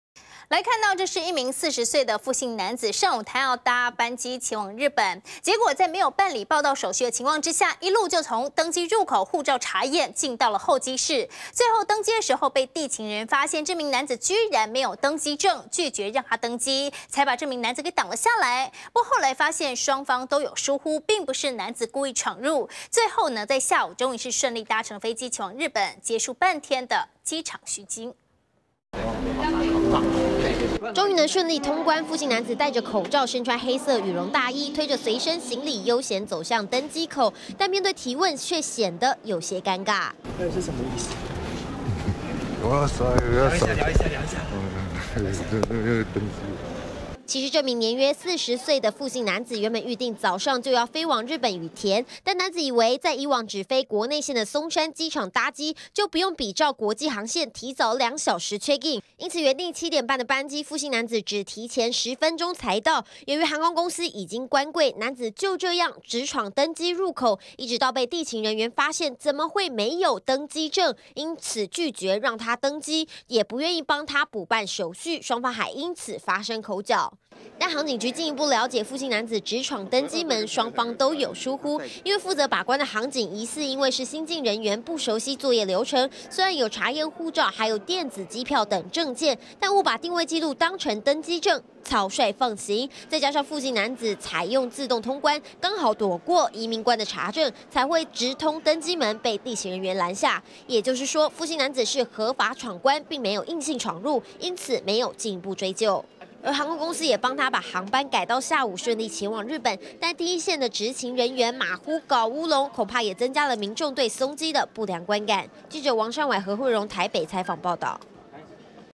20141204中天新聞 松機疏失！男無登機證竟直闖登機門
又發生機場闖關事件？一名40歲傅姓男子，上午要搭班前往日本，在沒辦理報到手續情況下，一路從登機入口、證照查驗一路進到候機室，最後在要登機時，被地勤人員發現男子竟然沒有登機證，拒絕他登機，才把男子擋了下來，後來才發現雙方都有疏忽，並不是男子故意闖入的，最後男子在下午終於順利搭機前往日本，結束了半天的機場虛驚。